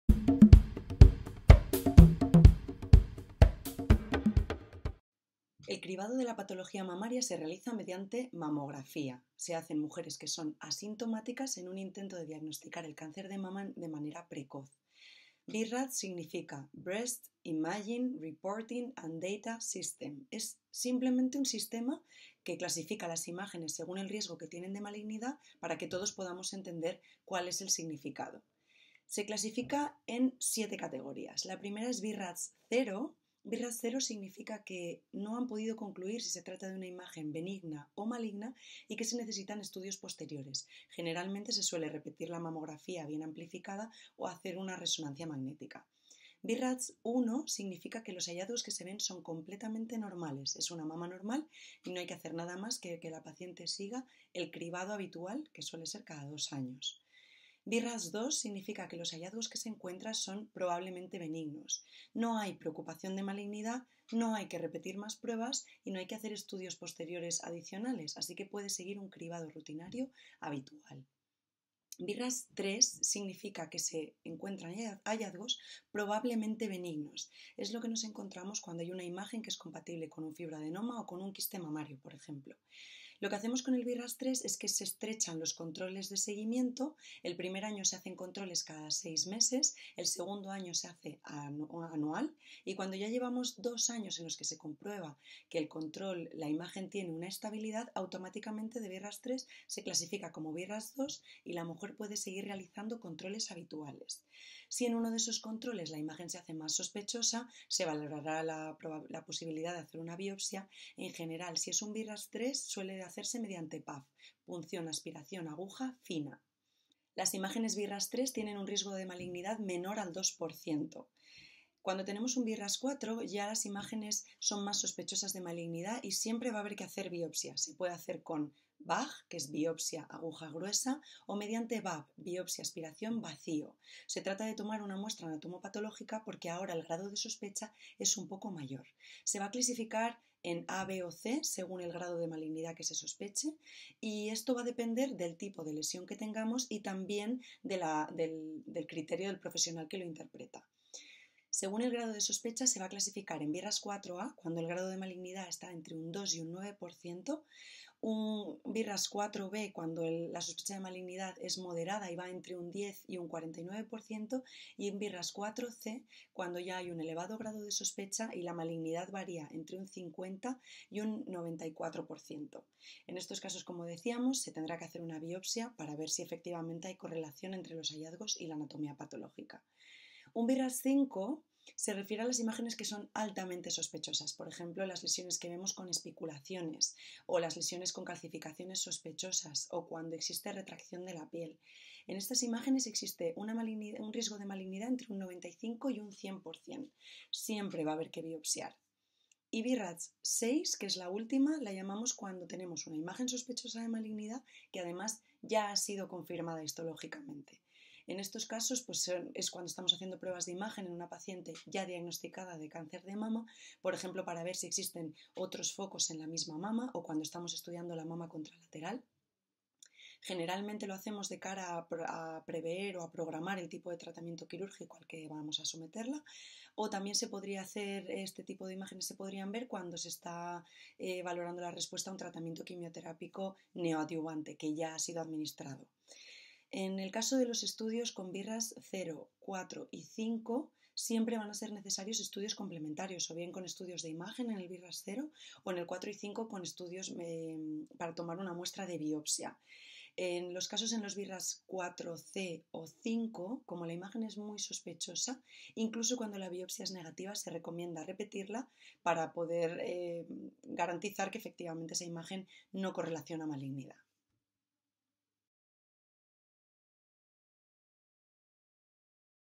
BIRADS: MAMOGRAFÍA y riesgo en función de la clasificación. - Ginecología y Obstetricia -
La escala BIRADS sirve para clasificar las mamografías en función del riesgo que representa cada una. La Dra Melgar nos explica esta clasificación, así como el manejo en cada uno de los casos.

Más información, vídeos, podcast y PDF en. AULAGINECOLOGIA.COM
(Síguenos también en Facebook, Twitter y en Instagram @aulaginecologia)

BIRADS
El cribado de patología mamaria se hace mediante mamografías en mujeres asintomáticas en un intento de diagnosticar el cáncer de mama en un estadío precoz.

Se clasifica en 7 categorías:

- BIRADS 0: examen incompleto. Se necesitan pruebas de imagen adicionales o mamografías previas para comparar. No es posible determinar una conclusión por la falta de información.

- BIRADS 1: exploración completamente normal. Puede continuar programa de cribado normal.

- BIRADS 2: los hallazgos encontrados son benignos, se encontraría por ejemplo cuando hay
fibroadenomas o quistes mamarios. No hay preocupación de malignidad y no se necesitan exámenes posteriores para confirmar. Se debe seguir cribado normal.

- BIRADS 3: hallazgos encontrados probablemente benignos. Tiene un riesgo de malignidad menor a un 2%. Cuando nos encontramos con esto, se realizan controles algo más estrechos hasta que se comprueba que son estables y no cambian con el tiempo. Se suele seguir cada 6 meses durante un año, después un control anual, y al cumplir el tiempo de dos años con la imagen estable, se clasifica como BIRADS 2, y puede continuar con cribado de rutina. Si en algún momento existe mayor sospecha, se planteará realización de biopsia, generalmente PAAF (punción aspiración
con aguja fina).

- BIRADS 4: sospechoso de malignidad, se debe considerar biopsia. Generalmente BAG (Biopsia aguja gruesa) o BAV (biopsia asistida por vacío). El grado de sospecha varía según el tipo de lesión y el profesional que lo interpreta. Según el grado de sospecha se clasifican en:
BIRADS 4a baja sospecha de malignidad (2-9%)
BIRADS 4b: moderada sospecha de malignidad (10-49%)
BIRADS 4c: alta sospecha de malignidad (50-94%)

- BIRADS 5: alta sospecha de malignidad (95-100%): siempre biopsiar. Son las lesiones con signos muy sospechosos como especulaciones, calcificaciones sospechosas o retracción de la piel.

- BIRADS 6: lesión sospechosa, cuya malignidad ya ha sido confirmada por una biopsia previa. Se
suelen ver en el contexto de imágenes realizadas en pacientes ya diagnosticadas, en las que se está estudiando si existen otros focos, o la mama contralateral, previo al tipo de tratamiento quirúrgico, o para valorar la respuesta a tratamiento quimioterápico neoadyuvante.

Los estudios con resultado BIRADS 0, 4 o 5, necesitan intervenciones complementarias (bien repitiendo pruebas de imagen, amplificando zonas, o mediante biopsia).

En los casos BIRADS 4c y 5, si la biopsia se informa como benigna, es conveniente plantearse repetir la biopsia.

Otros vídeos, podcast, PDF y mucho más:     AULAGINECOLOGIA.COM